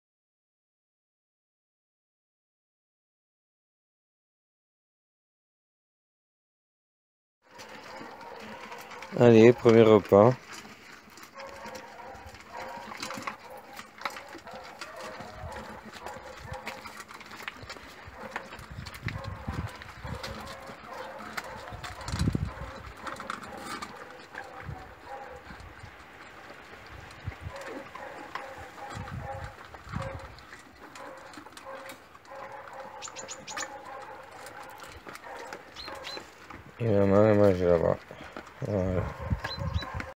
chiots Jade 2017 1° repas
le premier repas des chiots de Jade, Nioky toujours timide  commence a rentrer dans la gamelle